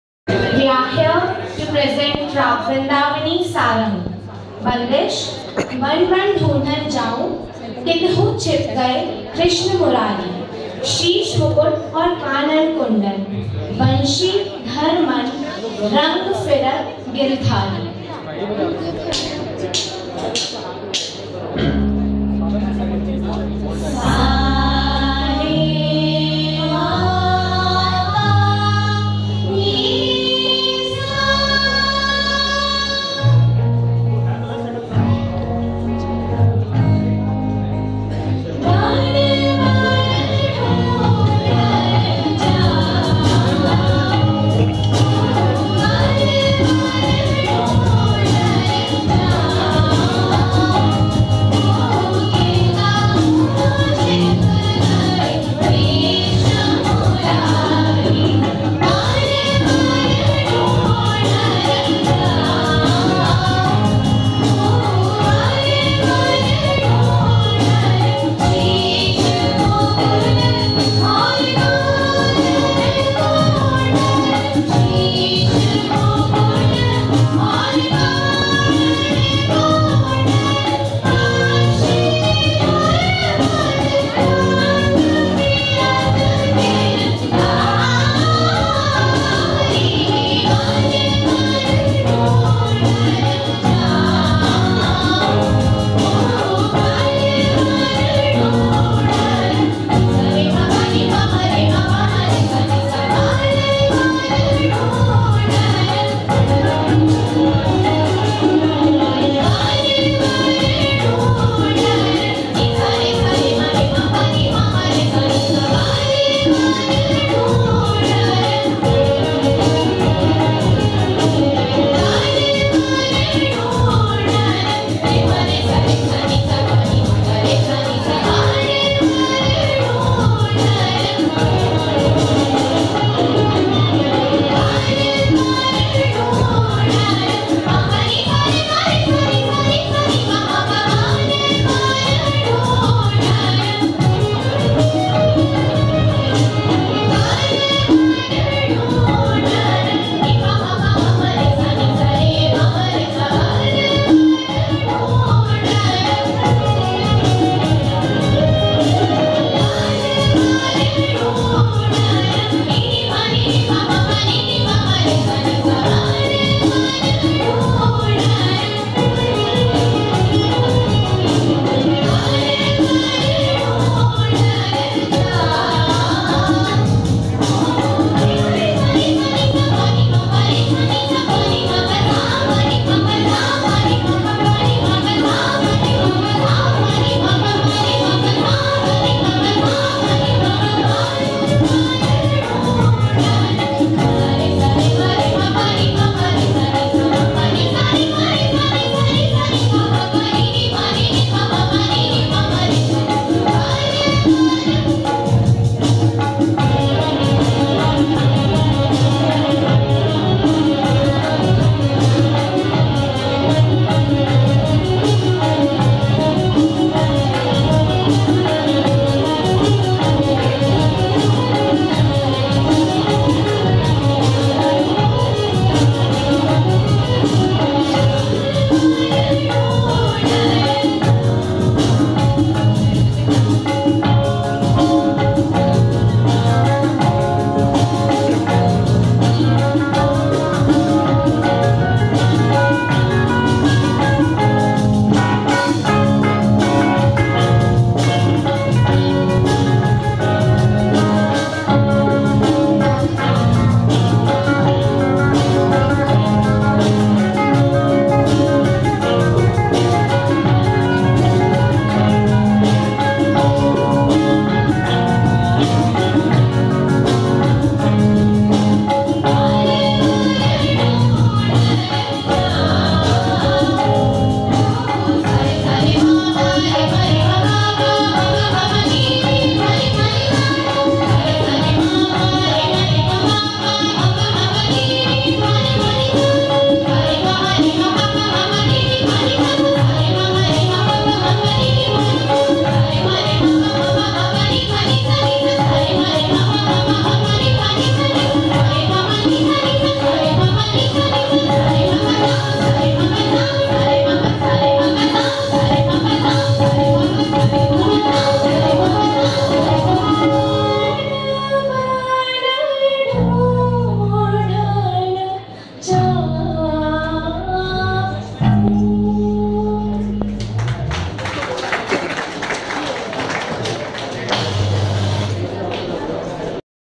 Classical Fusion Performance by D.A.V. Public School, Sahibabad
Classical Fusion compition performed by D.A.V. Public School, Sahibabad. Won 1st Prize
Students of Srikanth sir and Surya sir !!!